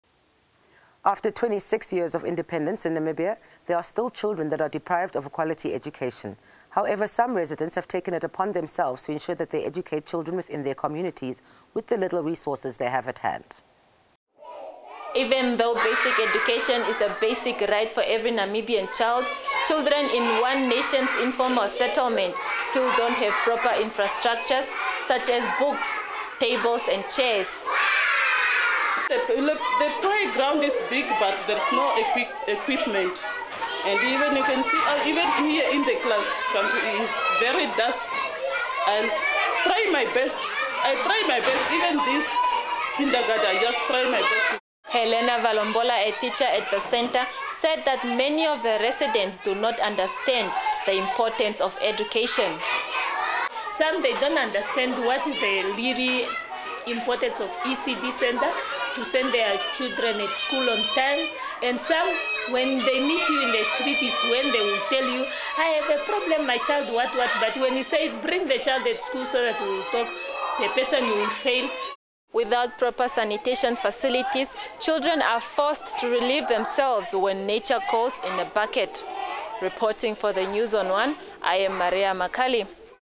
Adventist Development and Relief Agency donates winter stuff to Moses Garoeb residents
by: Rinelda Mouton ---
With the high rate of unemployment and poverty in the country, many within the informal settlements find it hard to stay warm during winter. As part of supporting the government's plan of rooting out poverty the Adventist Development and Relief Agency in Namibia  donated winter clothes and blankets to the residents in the  Moses Garoeb Constituency.